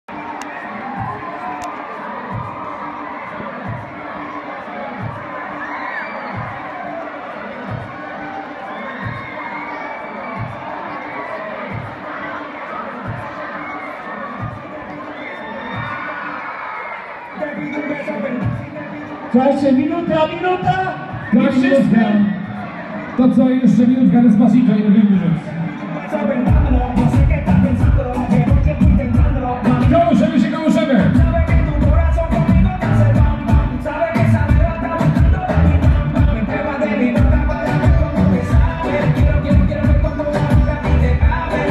Jastrzębie Zdrój: Eksplozja mocy 2017 Rozdawanie kolorów ze sceny
Krótki fragment imprezy ze stadionu miejskiego